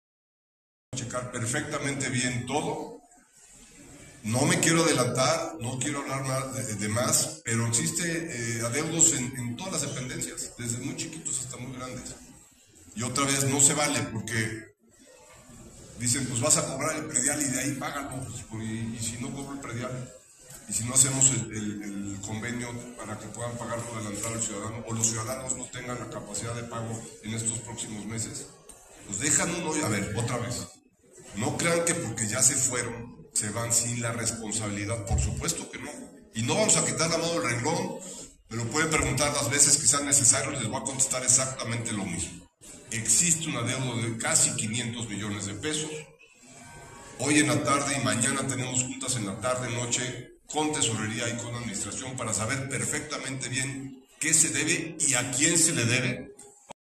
El edil de Puebla menciona que todas las dependencias del ayuntamiento tienen adeudos
El presidente municipal de Puebla, Pepe Chedraui, menciona que todas las dependencias del ayuntamiento tienen adeudos. El hoyo financiero es de alrededor de 500 mdp.